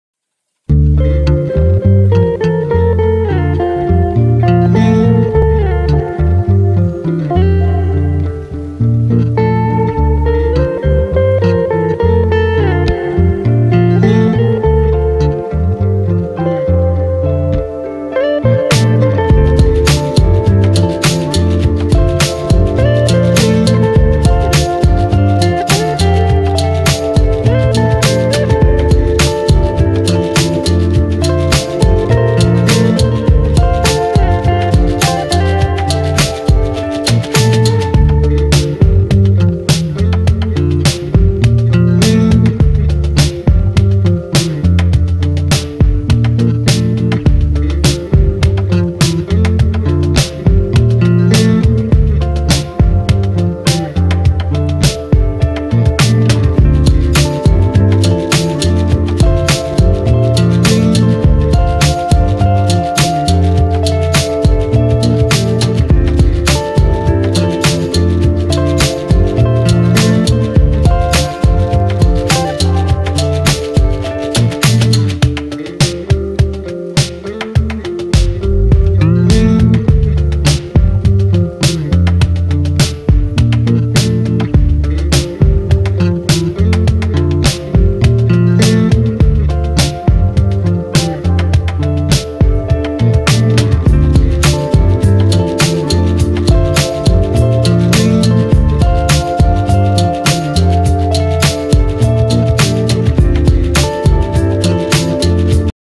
carp fish pond
This is a Carp fish pond from the spring fresh water from the mountain. This pond doesn't use any filter or chamber filter. It's located in the West Java, Indonesia, exactly in Cilember waterfall.